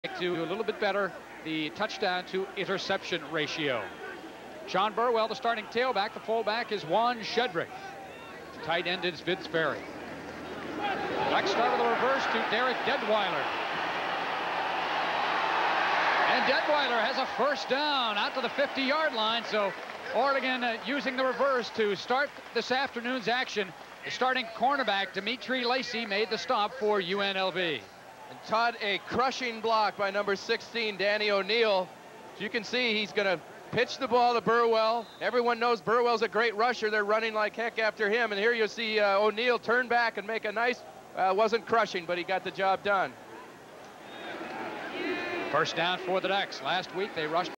Oregon WR Derrick Deadwiler runs a reverse for a first down vs. UNLV 9-26-92
Oregon WR Derrick Deadwiler runs a reverse for a first down, thanks to a "crushing...well it wasn't crushing" block by QB Danny O'Neil vs. UNLV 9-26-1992.